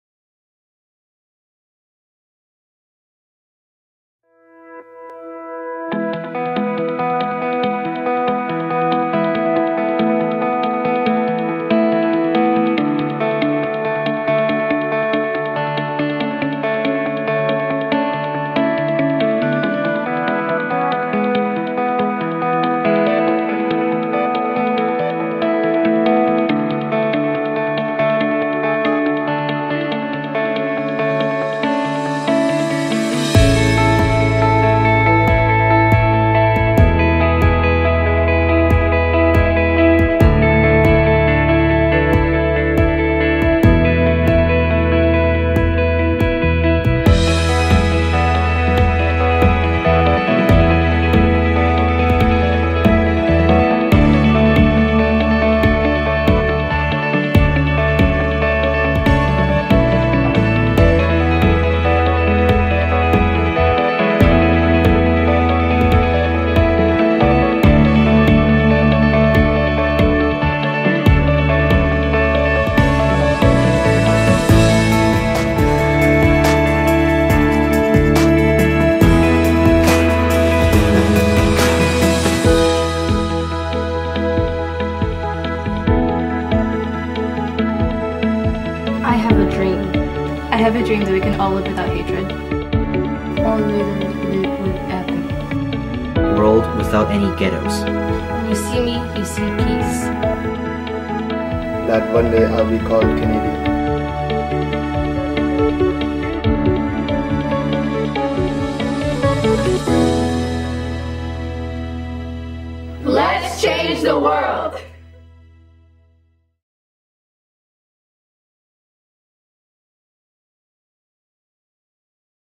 2017-2018 Anti-Racism Video: Clarke Road Secondary School
This video is recorded in Clarke Road Secondary School.
Let's stop racism.

LONDON, ON
CANADA